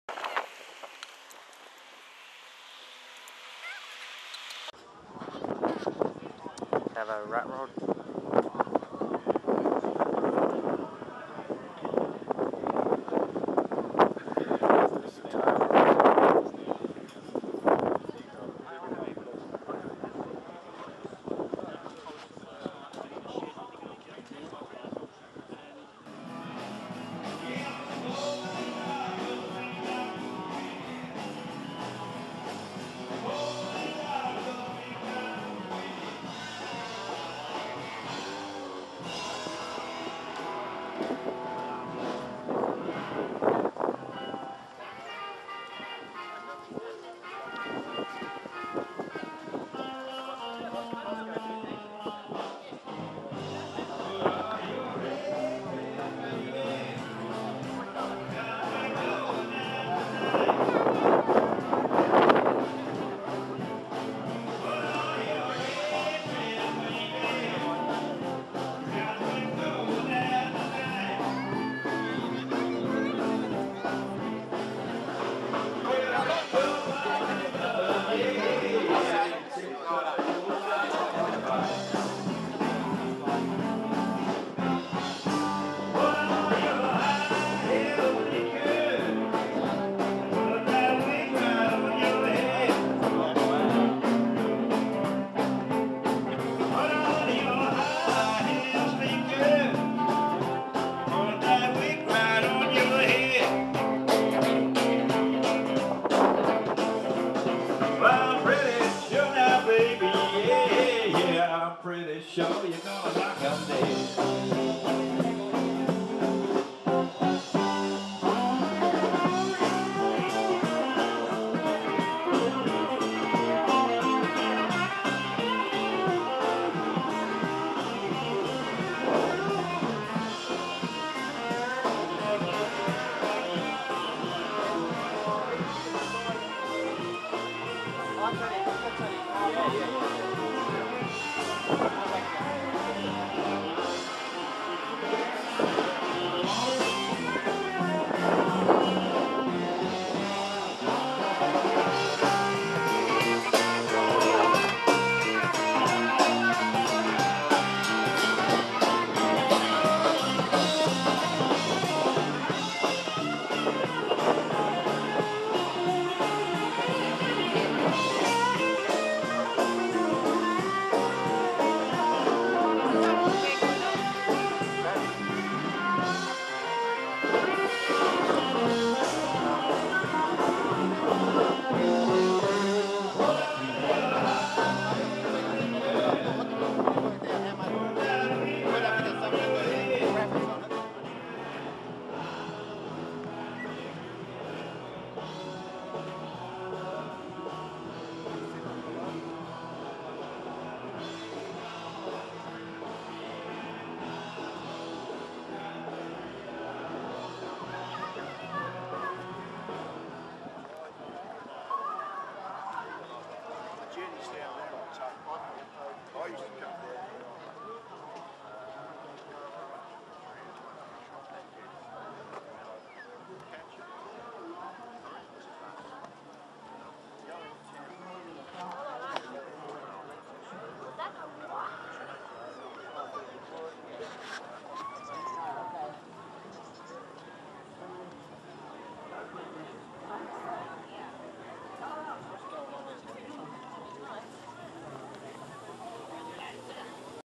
Volksfest Volkswagen Festival by Volks Enthusiasts Club of South Australis
Now may i first apoligise for that bumpy camera work, i will remember to take more care. (i had a really long video but it made me motion sick :P, so this is all you get).
It was a nice turn out but the chance of rain kept most of the trailer queens away, pretty cool Rat Rod as well. But if you want to see some pictures from the day check my

Enjoy all and take it easy.